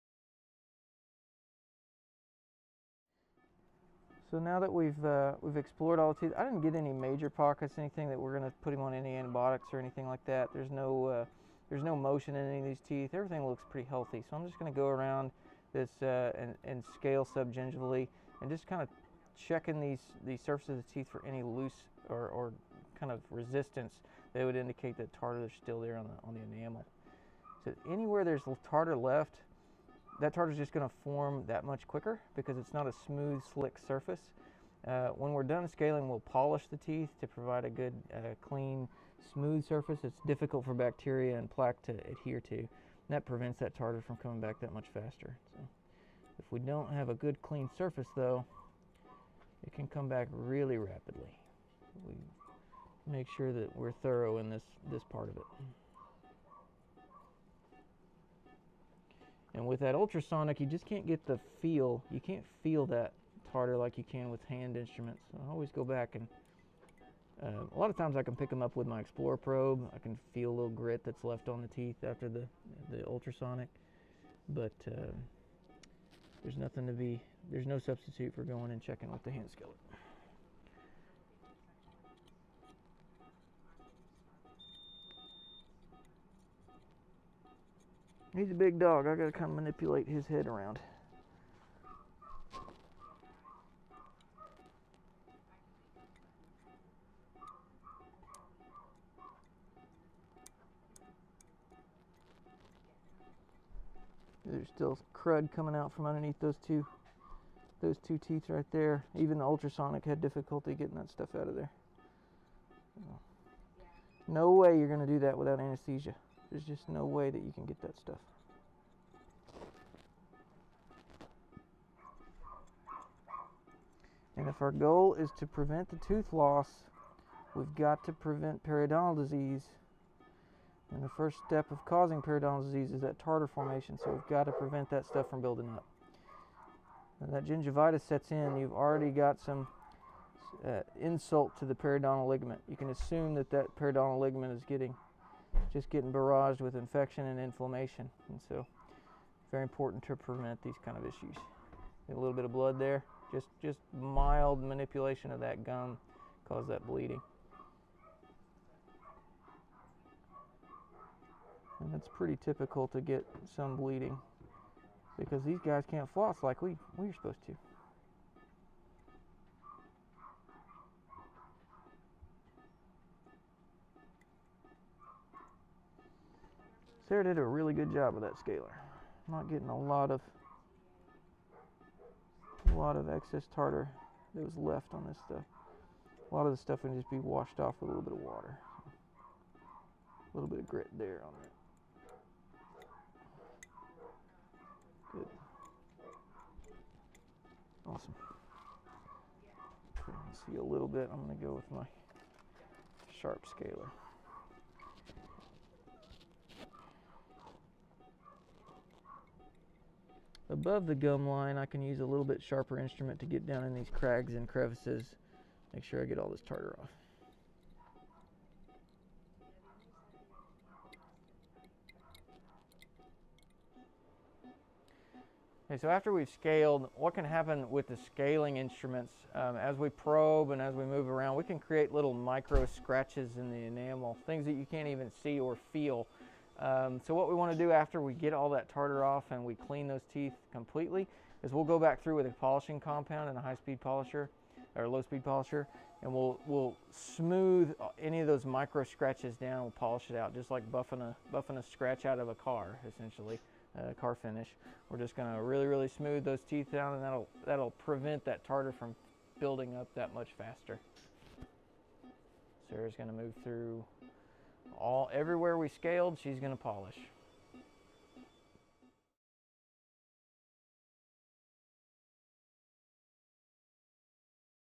Dental Procedure Series, Episode 8: Hand Scaling & Polishing
Join us as we take you step by step through a Dental Procedure, in honor of National Pet Dental Health Month! "Like" and follow our facebook page for more!